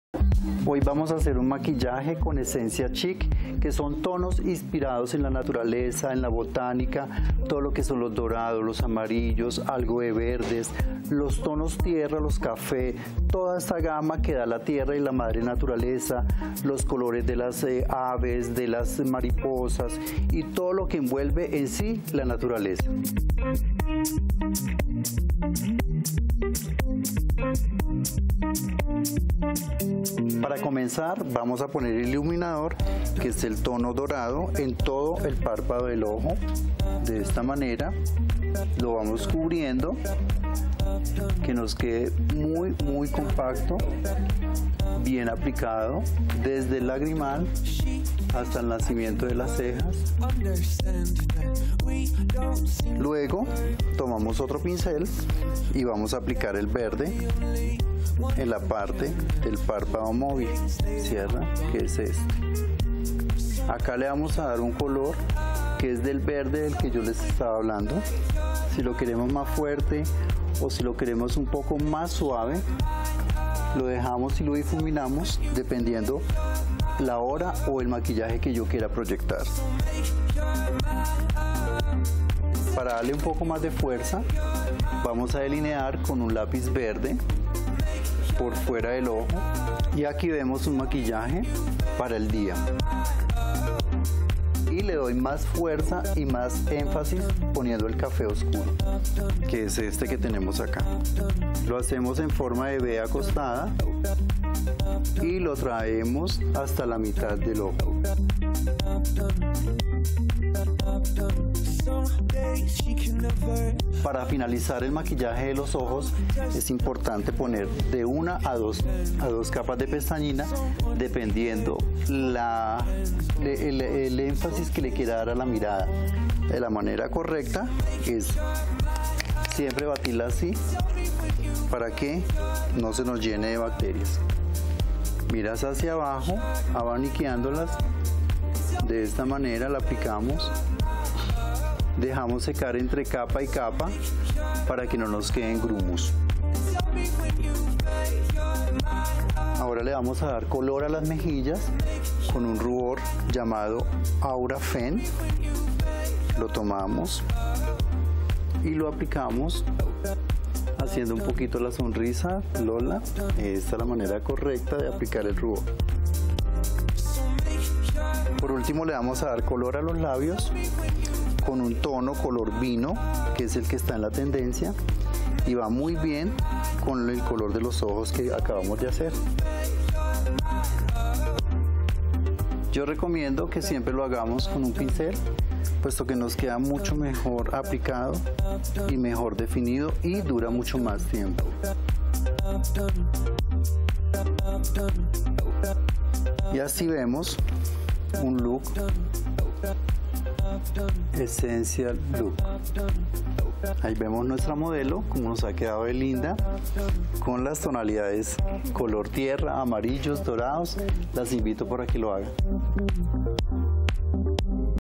Maquillaje con tonos inspirados en la naturaleza
Los colores de aves, de la tierra, de los animales y las flores son la inspiración para este tipo de maquillaje. Aprende más viendo el video.